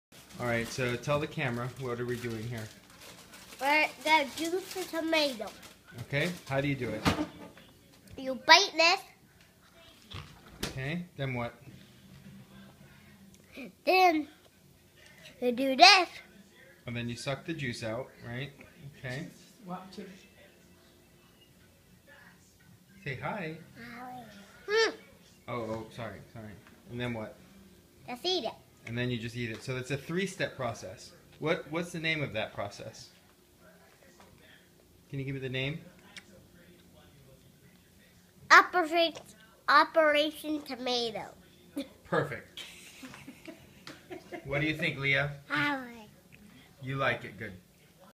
Operation tomato
Luke explains to us how you're supposed to eat a cherry tomato